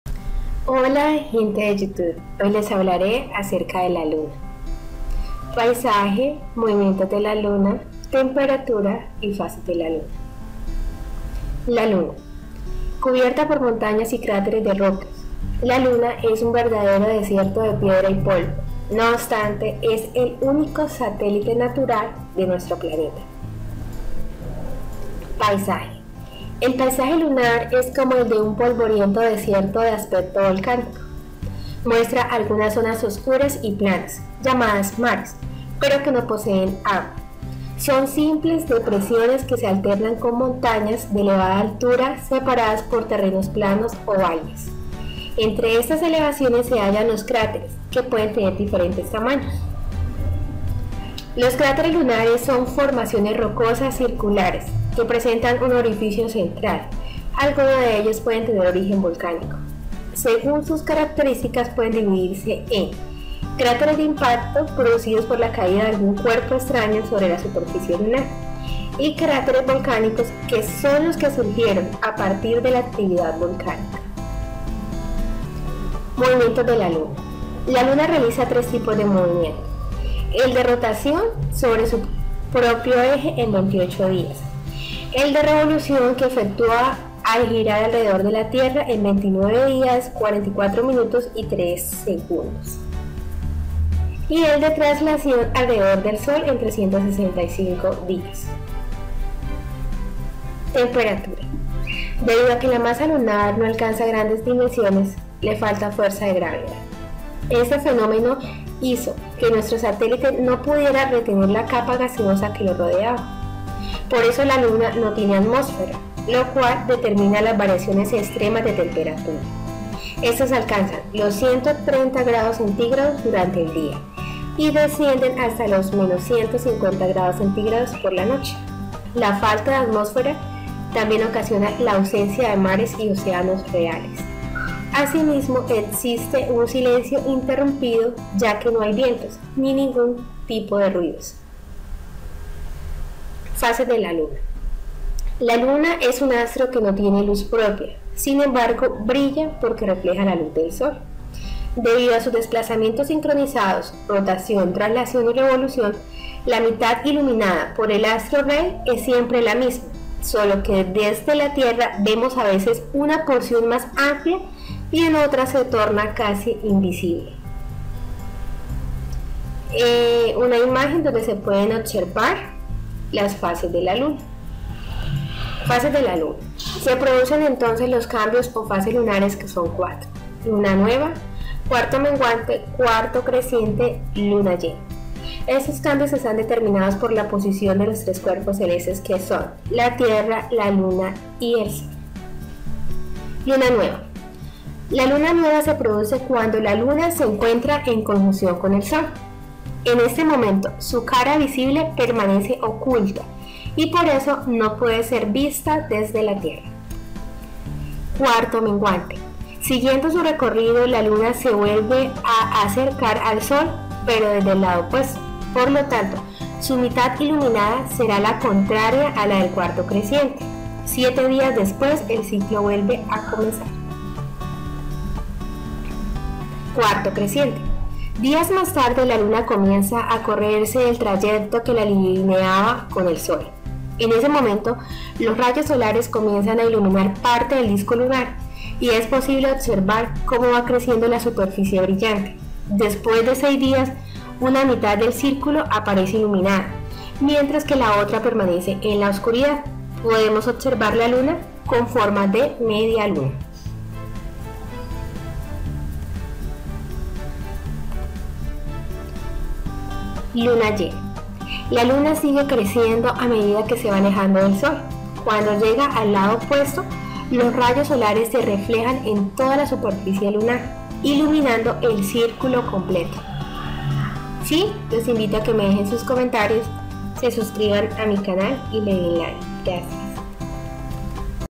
Cúales son las Fases de la Luna* movimientos de la Luna.*Luna🌙✔
TODO sobre LA LUNA  y sus 4  FASES : luna llena,  luna  nueva, cuarto creciente , cuarto menguante).

💙Si te ha gustado este vídeo, COMPÀRTELO Y SUSCRÌBETE sin costo a mi CANAL jopabaa

Gracias 🌈

Un saludo  para todos.